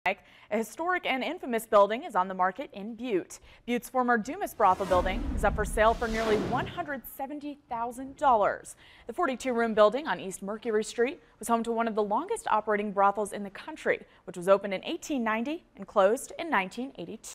Historic Dumas Brothel up for sale
Butte's former Dumas Brothel building is up for sale for nearly $170.000. The 42-room building on East Mercury Street was home to one of the longest operating brothels in the country, which was opened in 1890 and was closed in 1982.